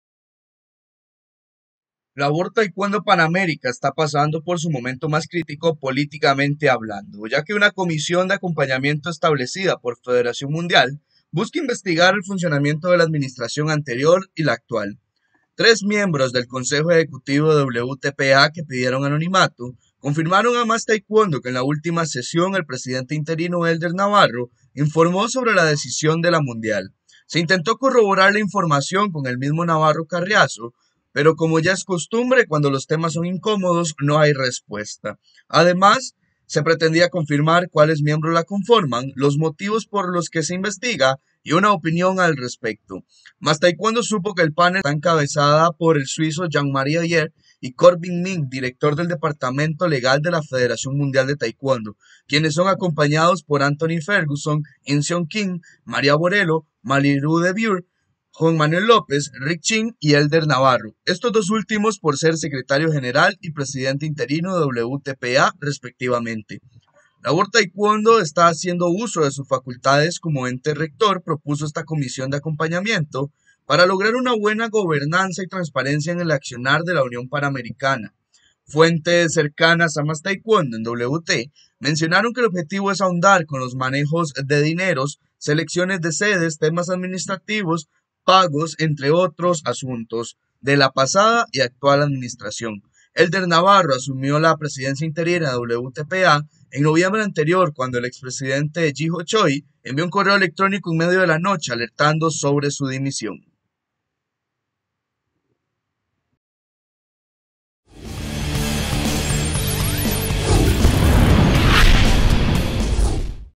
WT instaura comisión normalizadora para investigar WTPA
Federación Mundial instauró una Comisión Normalizadora para investigar la pasada y actual administración de la World Taekwondo Pan América.